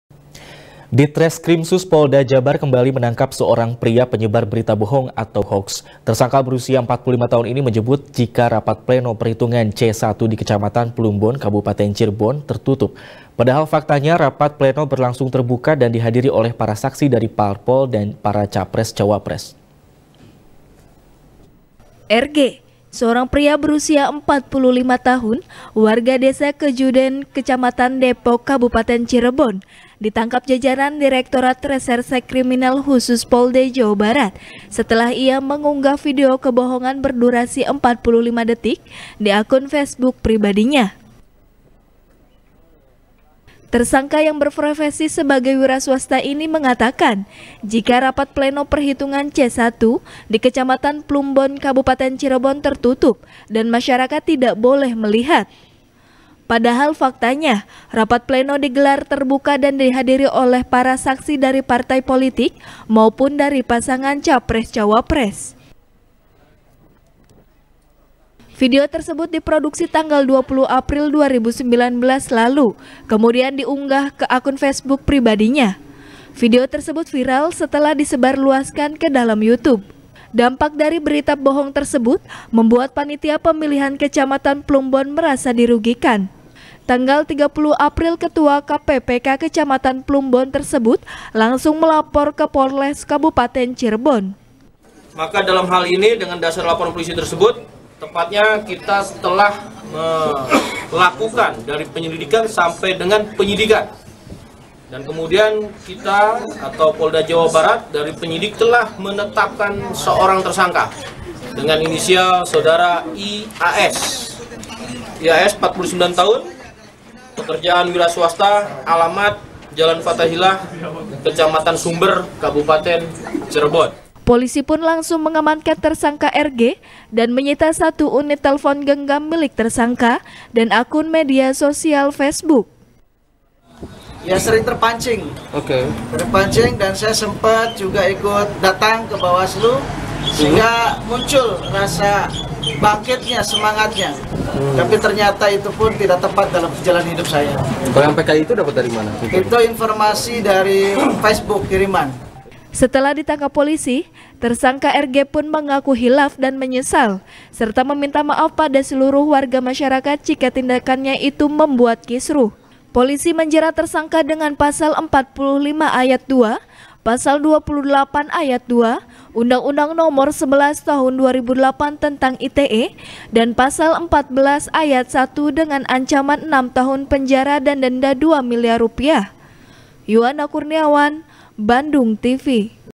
Lagi, POLDA JABAR Bekuk Pelaku Penyebar Berita Bohong | SBR | BANDUNGTV
SBR | BANDUNGTV | IRFAN RAMANDA | Ditreskrimsus Polda Jabar, Kembali Menangkap Seorang Pria,Penyebar Berita Bohong Atau Hoax.Tersangka  Berusia 45 Tahun Ini Menyebut,Jika Rapat Pleno Perhitungan C Satu Di Kecamatan Plumbon,Kabupaten Cirebon Tertutup.Padahal Faktanya,Rapat Pleno Berlangsung Terbuka,Dan Dihadiri Oleh Para Saksi Dari Parpol Dan Para Capres Cawapres.